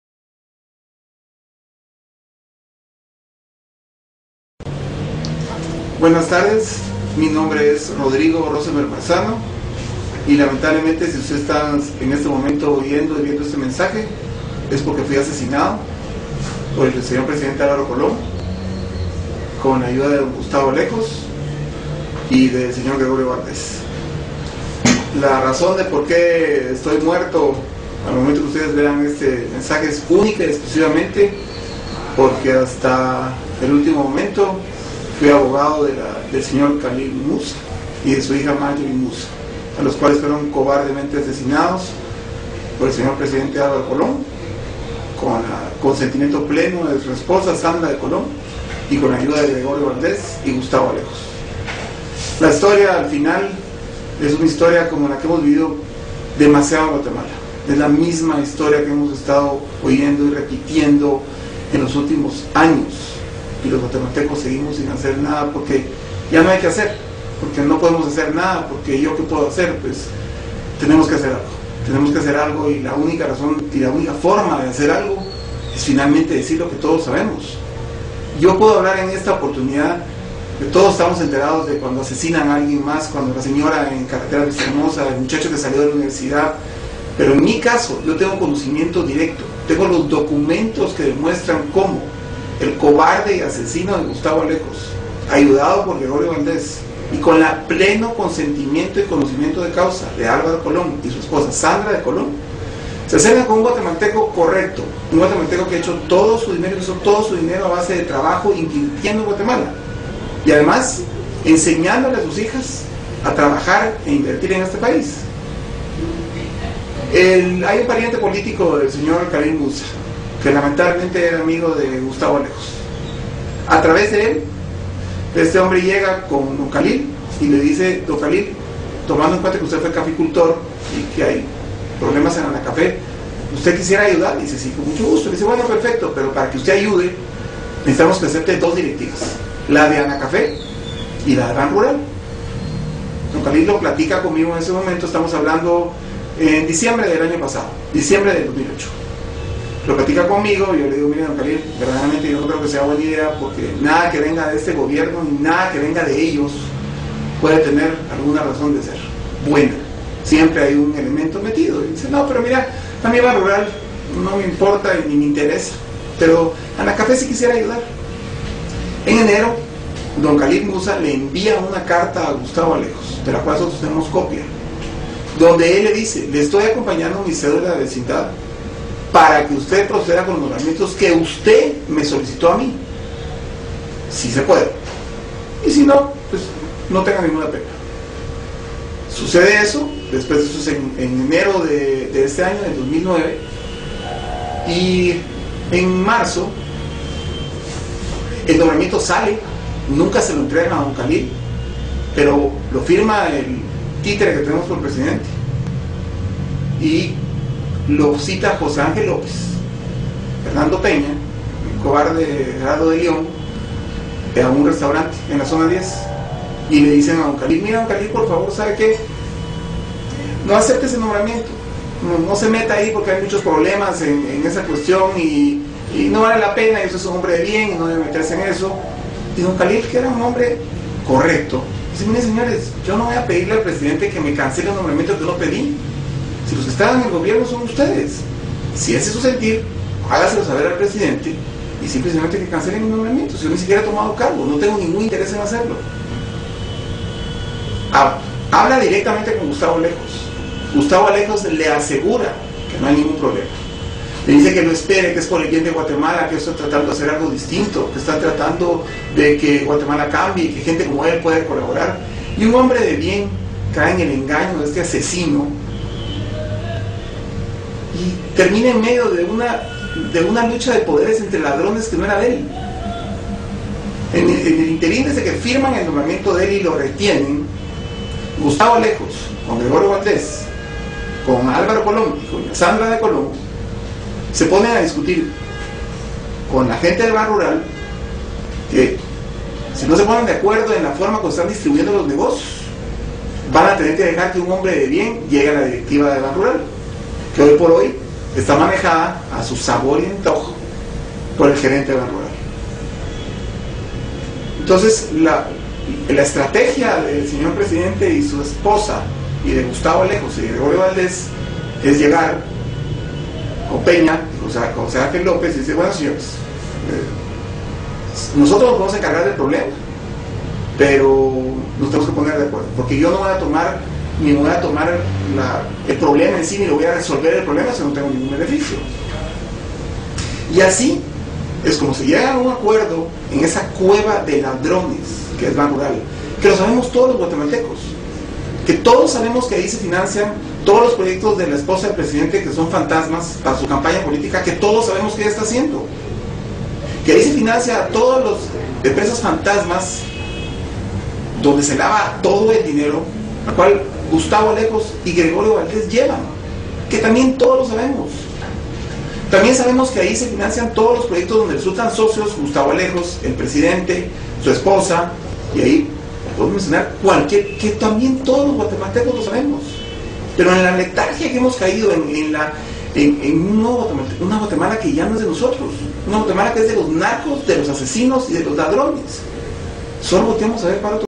Rodrigo Rosenberg Asesinado , Video 1 del porque lo Mataron English
Good Afternoon
My Name is Rodrigo Rosenberg Manzano and If you are reading this message it is because I have been murdered by Gustavo Alejos, the President´s Private Secretariat and his partner Gregorio Valdez, with the approval of Alvaro Colom (The President) and Sandra de Colom (The First Lady).

The reason why Gustavo Alejos and Gregorio Valdez have ordered my death and the President of the Republic, Alvaro Colom approved it, is because until the day they killed me, I was the lawyer of two incredible Guatemalans, Mr. Khalil Musa and his daughter Marjorie Musa, and I knew exactly how Alvaro Colom, Sandra de Colom, Gustavo Alejos and Gregorio Valdez were responsible for that cowardly murder, and I told them so and told those who wanted and could hear.

I was a 47 year old Guatemalan, with 4 divine children, with the best brother one could ask life for, with incredible friends, who wanted very much to live in my country, but I could not have lived with myself without rebelling, being brave and denouncing to all Guatemalans that have principles and values about the real reasons for the death of Mr. Kahlil Musa and his daughter Marjorie Musa, with no regards to the consequences and in the understanding that my life was in danger I wanted to leave this testament in case something happened to me and it unfortunately did.

Alvaro Colom, by means of Gustavo Alejos and Gregorio Valdez asked Mr. Khalil Musa for his collaboration so he would form a part of the board of directors of Banrural in an ad honorem fashion, Mr. Khalil Musa did not suspect of the millionaire illegal businesses that are done every day in Banrural, those businesses go from money laundering to deviating public funds to inexistent programs of the first lady Sandra de Colom, as well as financing to shell companies used by drugtraffickers.

It is Alvaro Colom in agreement with Gustavo Alejos and Gregorio Valdez that retain the already signed appointment of Mr. Khalil Musa, without him knowing about it, for more than 3 months, because they did not have any intention of having him assume as Director, they only used his good name with the argument that if new power cuotas were not distributed Mr. Khalil would denounce, when he discovered, the corruption with which the general manager of Banrural, Mr. Fernando Peña, manages the bank at his own pleasure to the service of Mrs. Sandra de Colom and as a partner and financier with the bank´s funds of the businesses of Gregorio Valdez and Gustavo Alejos, without the President of the Bank, Mr. Jose Angel Lopez doing anything to prevent that Fernando Peña converts Banrural into the cave of robbers, drug traffickers and murderers it is today.

With the impunity that we Guatemalans have granted in the past years to the robbers and murderers in this country, Jose Angel Lopez, Fernando Peña and the coward Gerardo de Leon, in a direct fashion threaten and intimidate Mr. Khalil Musa, a few weeks before his and his daughter´s murder, so that he resigns his appointment and Mr. Khalil Musa tells them, as the gentleman he always was, that he had no problem if the appointment was cancelled, since whom asked him to accept the appointment was Gustavo Alejos and Gregorio Valdez in agreement with our splendid president Alvaro Colom and his permanent shadow Sandra de Colom.